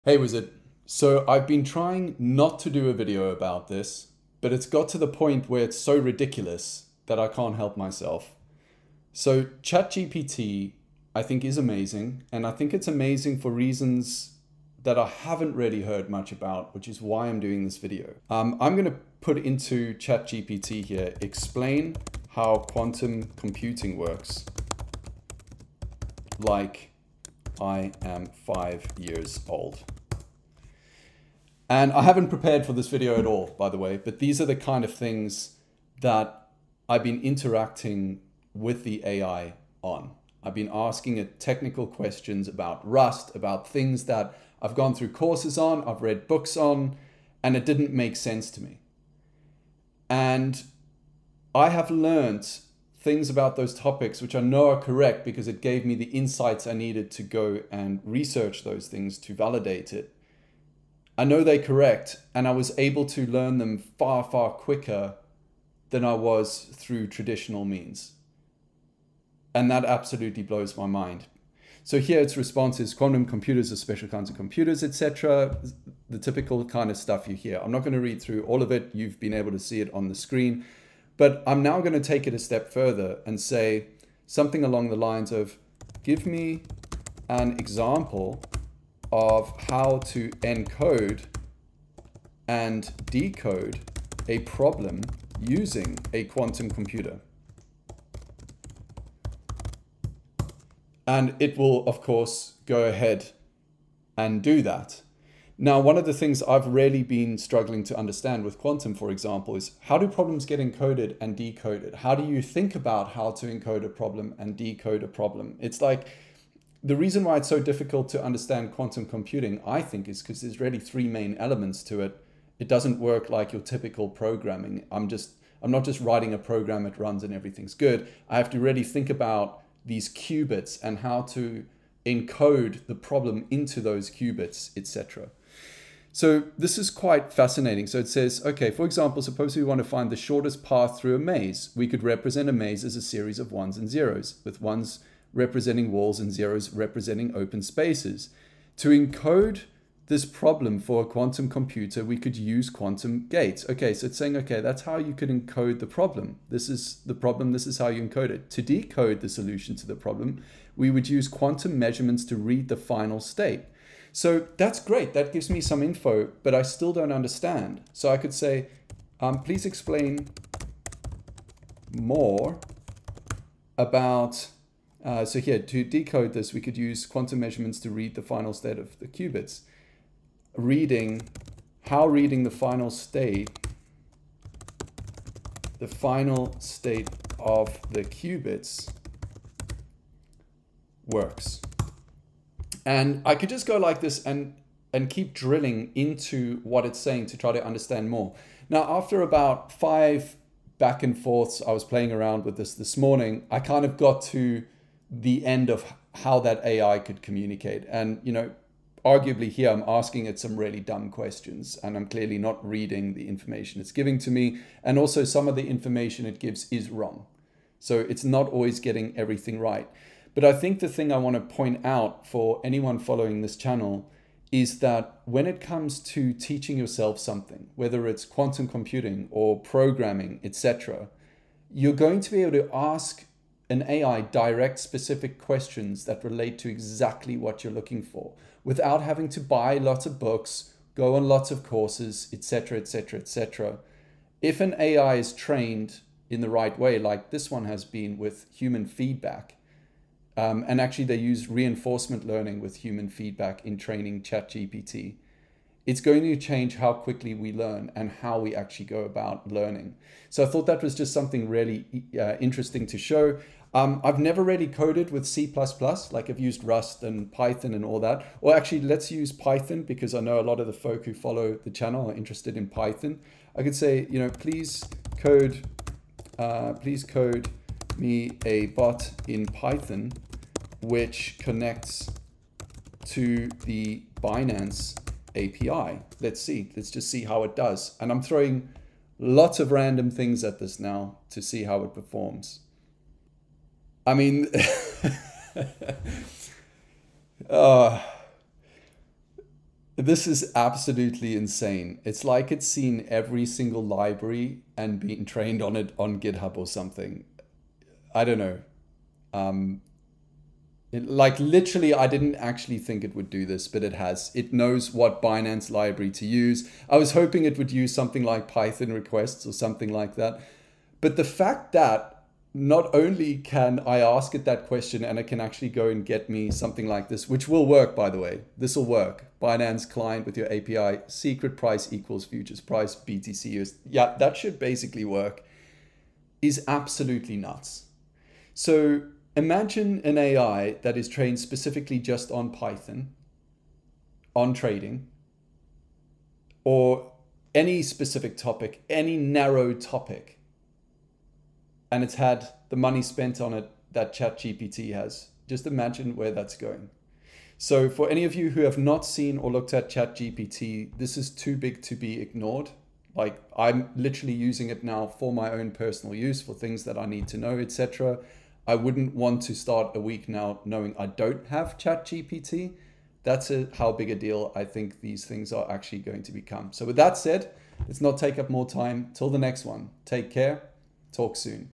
ChatGPT Will Change How We Teach Ourselves Technology
ChatGPT has an uncanny ability to present and summarise information tailored specifically in the way you request it. Ask it how to program something in RUST or perhaps like in this video, about how quantum computers work and you will not be disappointed.

Nothing here in this video except just some insight into how I use ChatGPT to help explain specific questions to me. If you are a coder - this tool is incredible for learning programming languages. Mind blown.